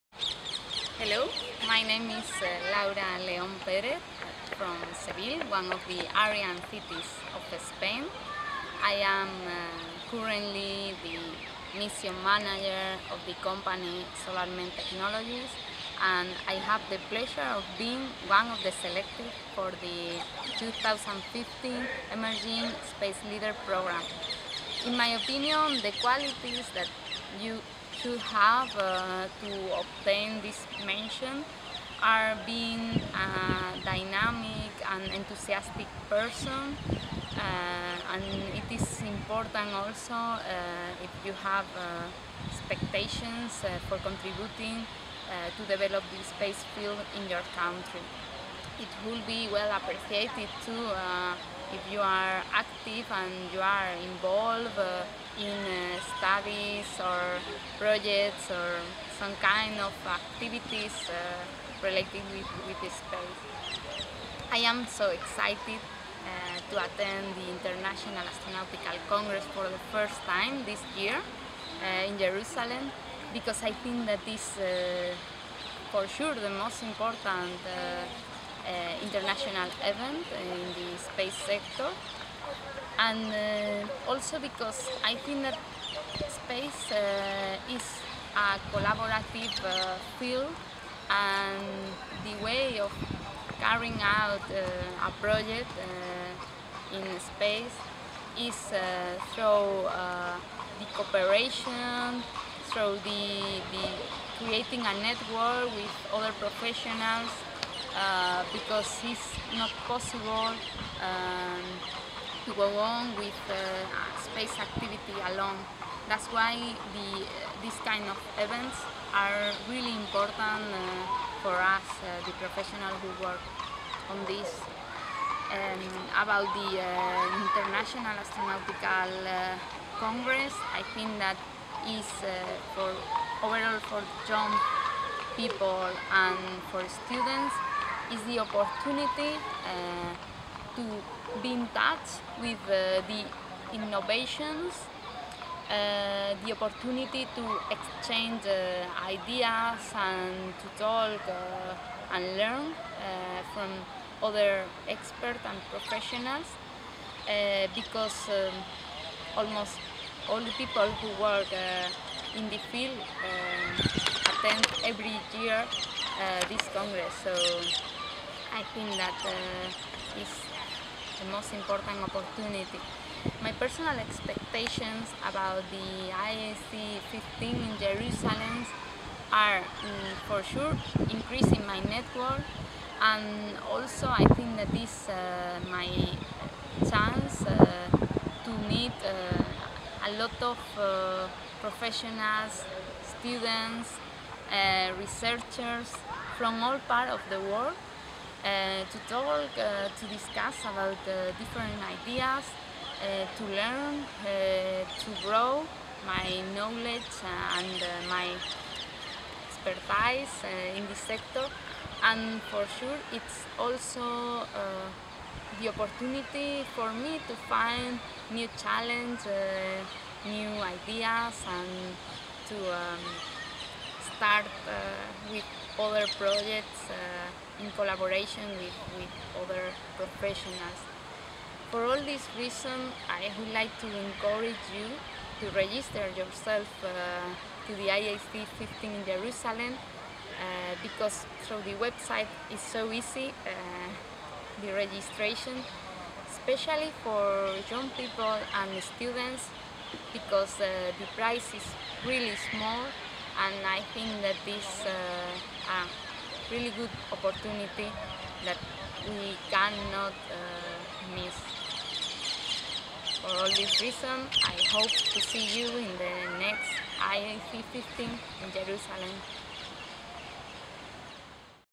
Emerging Space Leader IAC2015 Laura León Pérez
Laura León Pérez is one of the 2015 Emerging Space Leaders Winners and she will attend to the IAC'15 Jerusalem under this program with the presentation "Standarizing a simple and fast way to reach an orbit".